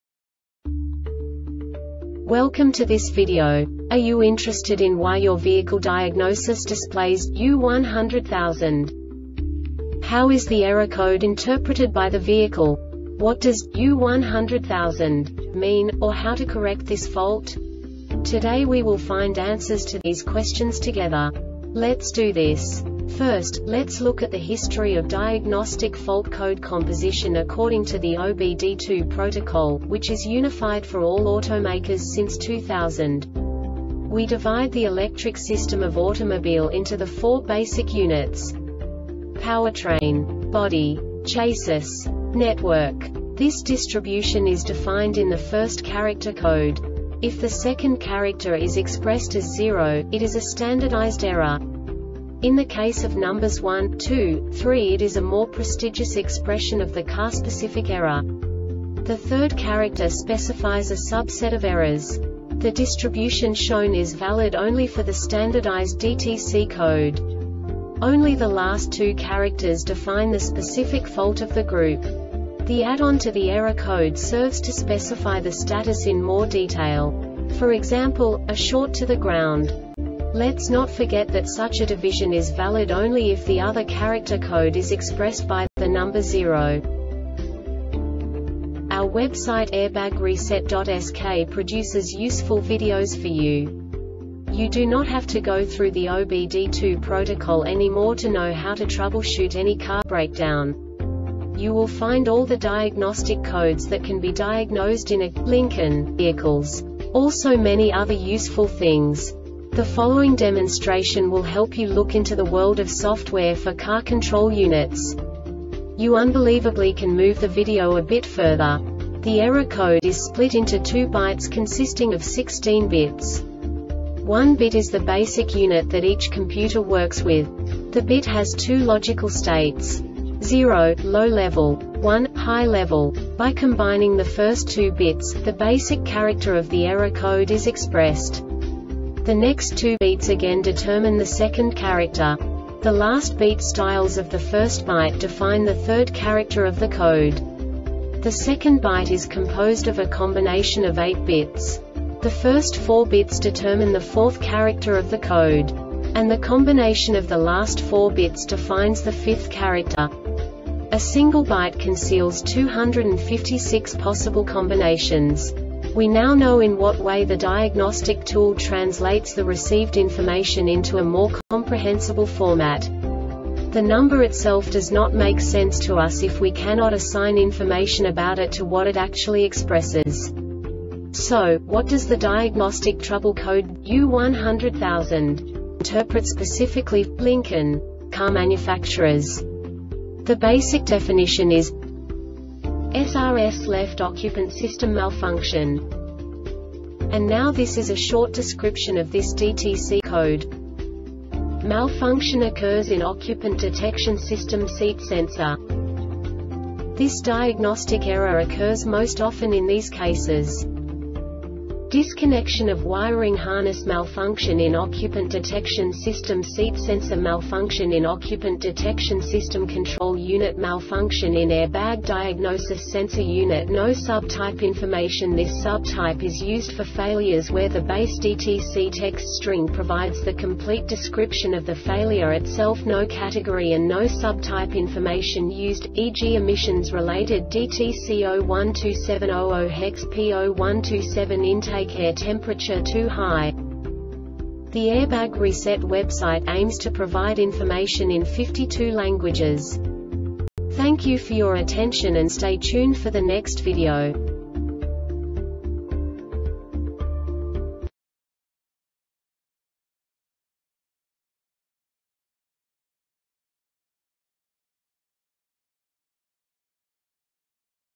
DTC Lincoln U1000-00 Short Explanation
Contents:
0:21 Basic DTC analysis according to OBD2 protocol standard.
1:48 Insight into programming
2:58 A diagnostically understandable description of the Diagnostic Trouble Code
3:05 Basic error definition

Make:
Lincoln
Code:
U1000 00
Definition:
SRS Left Occupant System Malfunction
Description:
Malfunction occurs in Occupant Detection System seat sensor.
Cause:
 Disconnection of wiring harness Malfunction in Occupant Detection System seat sensor Malfunction in Occupant Detection System control unit Malfunction in air bag diagnosis sensor unit
Failure Type: